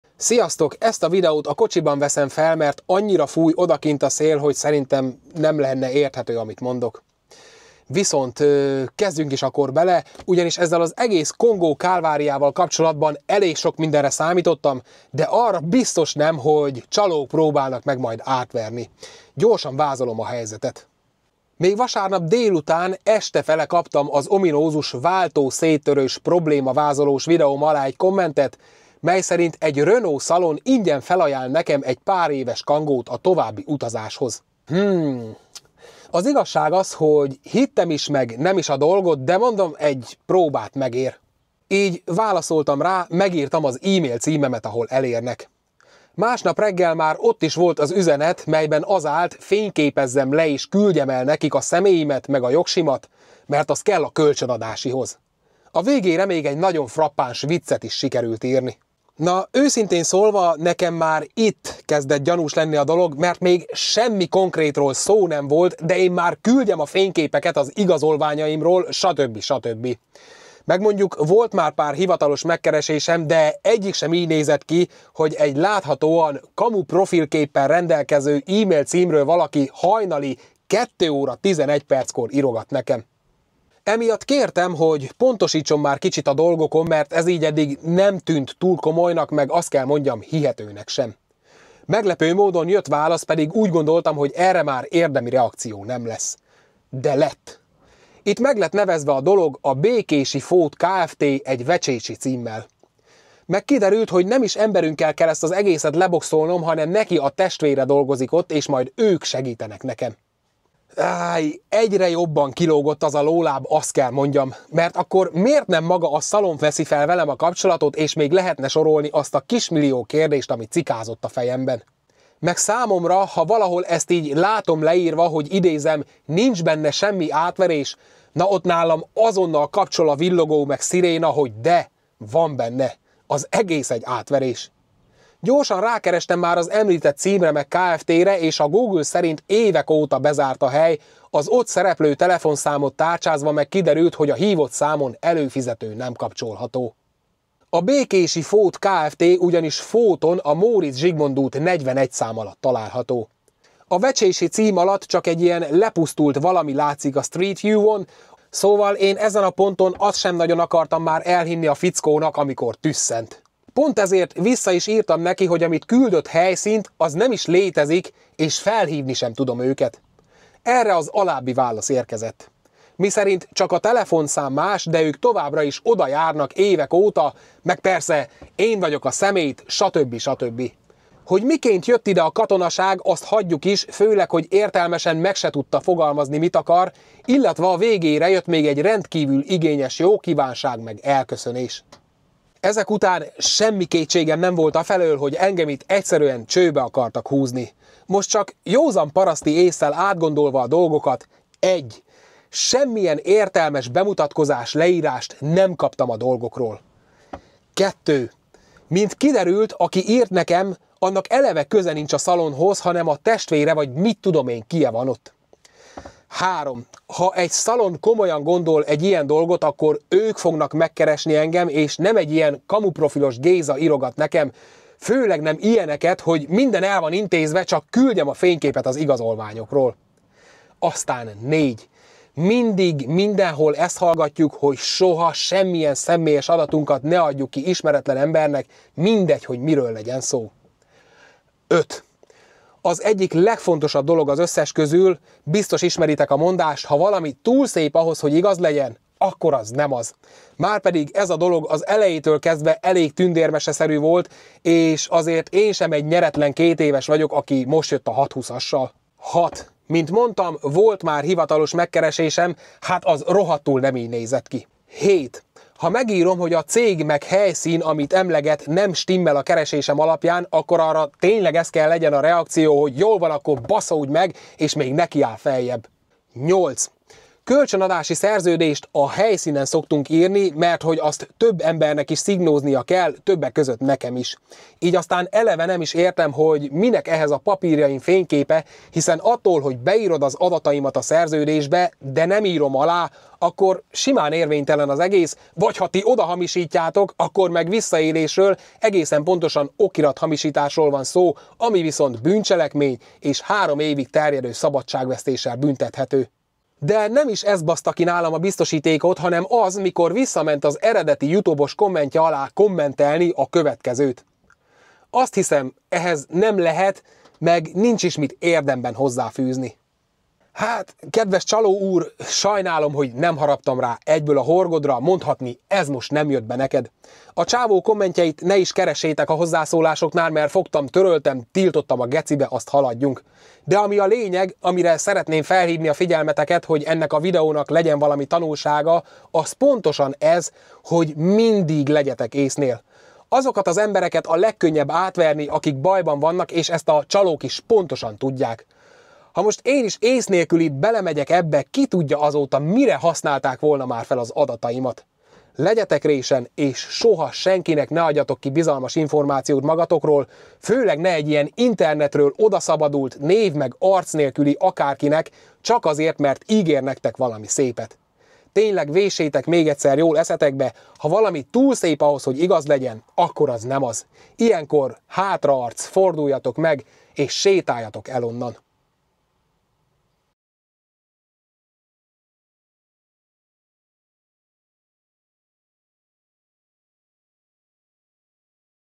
MEGTALÁLT EGY CSALÓ!!!
Sziasztok. Ezzel a Gongó kálváriával kapcsolatban elég sokmindenre számítottam,de arra biztos nem,hogy csalók próbálnak majd átverni. Gyorsan vázolom mi a helyzet.